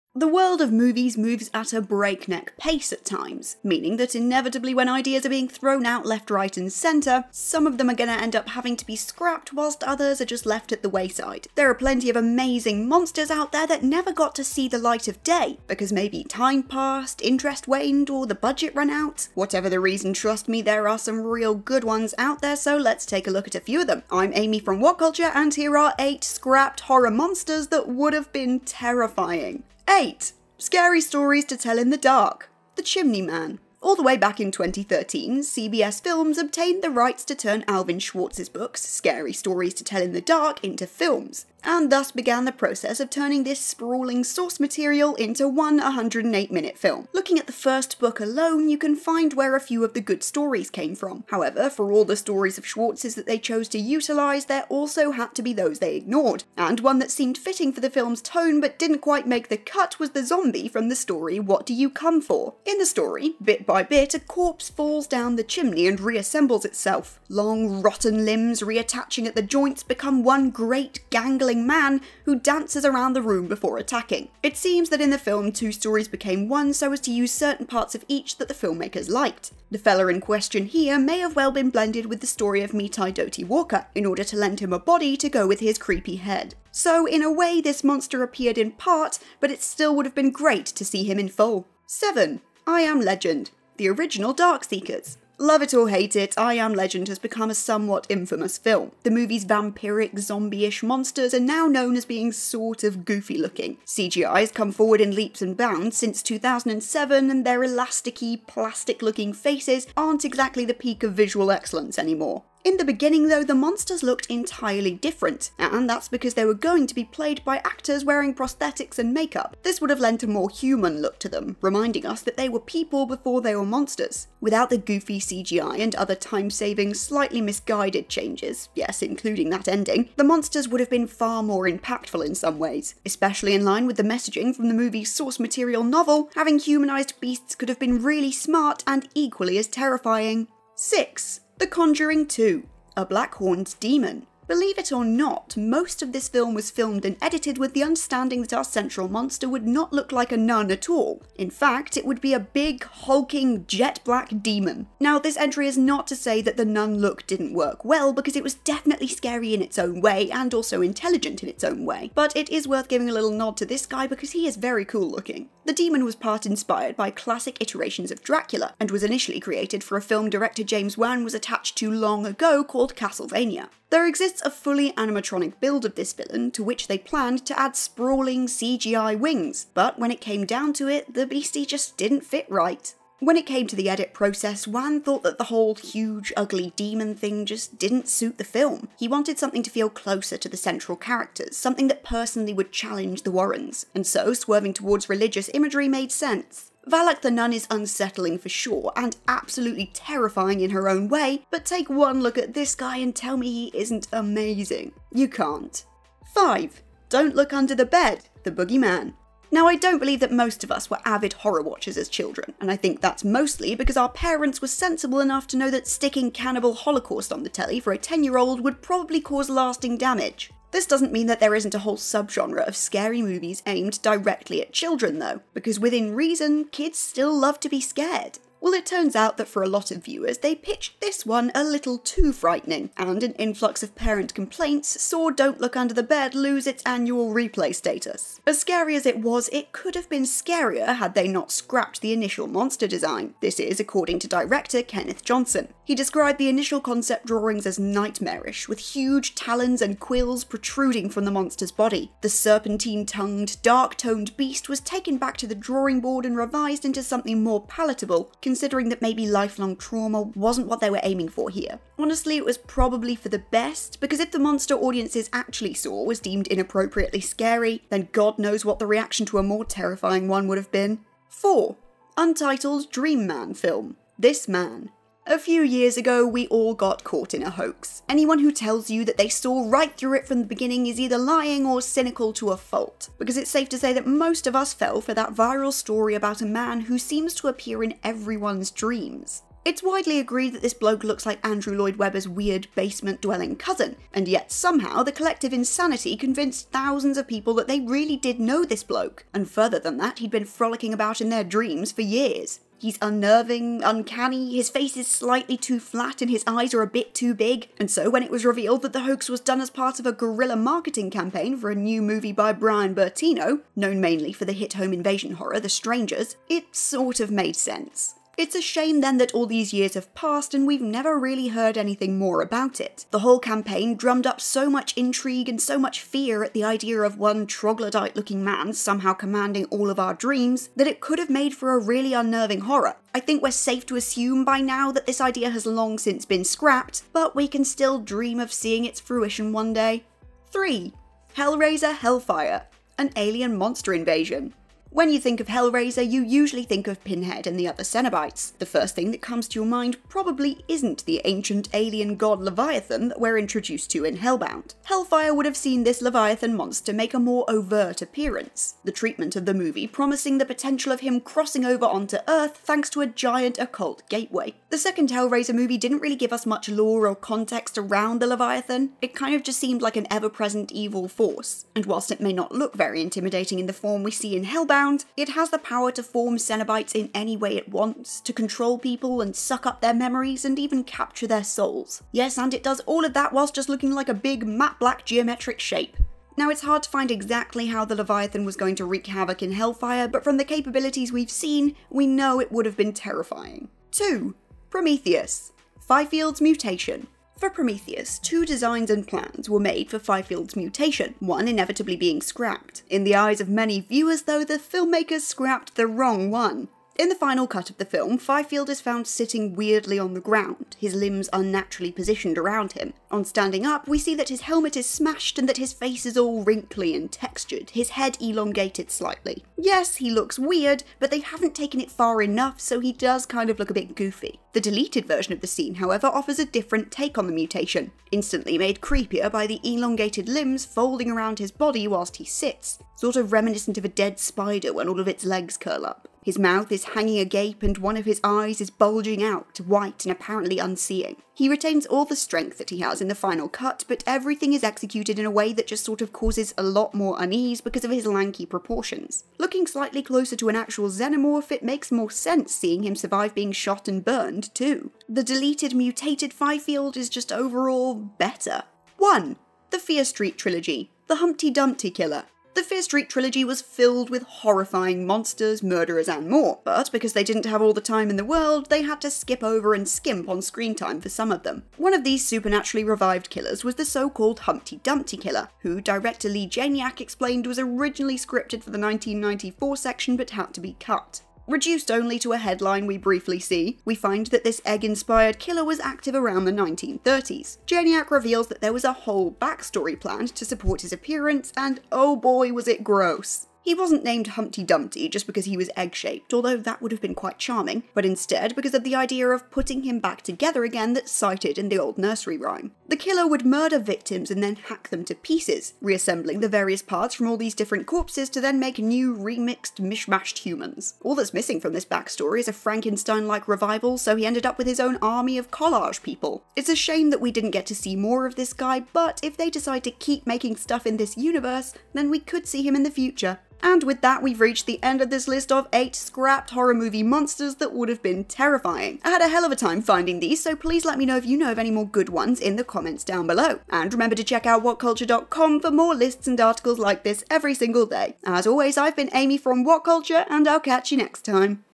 10 Scrapped Horror Monsters That Would’ve Been TERRIFYING
Horrifying horror monsters that you never got to see. Hellraiser, The Conjuring & more!